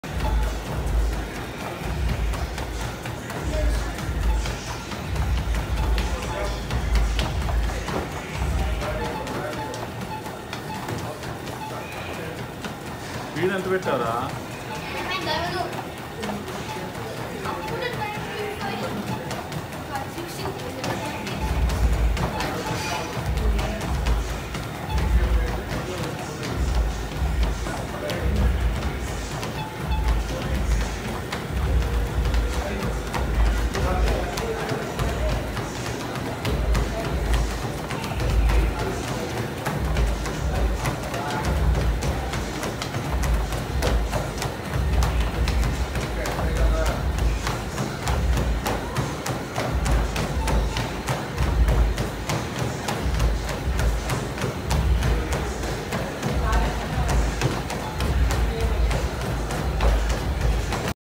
Vishnu and Manoj Morning treadmill workout
Hi all Vishnu and Manoj doing morning treadmill workout.